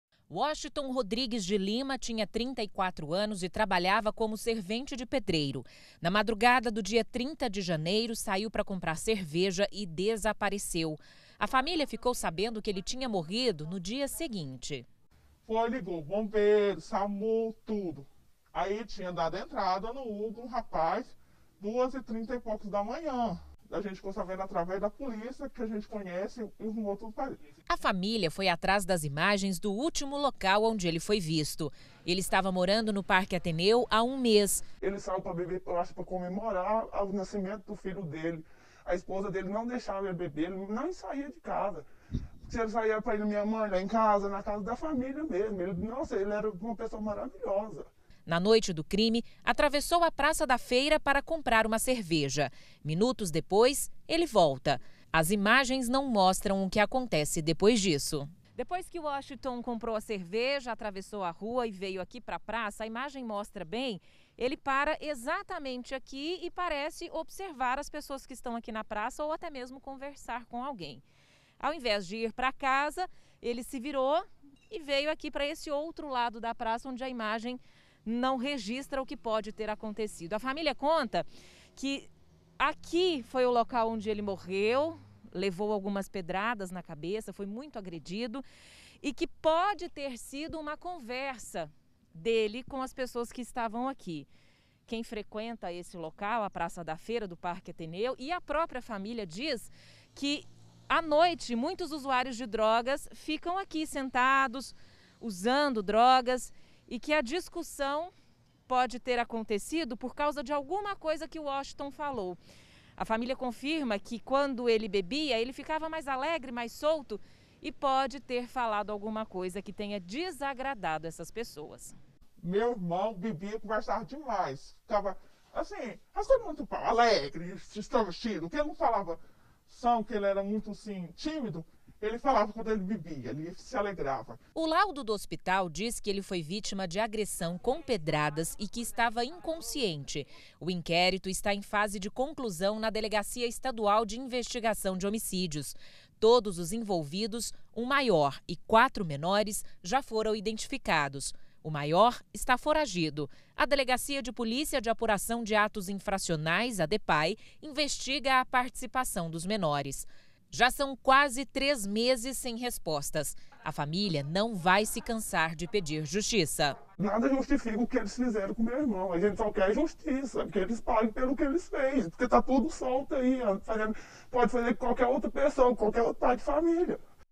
PROCURA-SE: IDENTIFICADOS ASSASSINOS DE HOMEM MORTO A PEDRADAS
QUASE TRÊS MESES DEPOIS, A POLÍCIA CIVIL ESTÁ PERTO DE CONCLUIR O INQUÉRITO SOBRE O HOMEM QUE MORREU A PEDRADAS NO PARQUE ATHENEU, EM GOIÂNIA. UM HOMEM E QUATRO MENORES QUE ESTARIAM ENVOLVIDOS NO CRIME JÁ FORAM IDENTIFICADOS.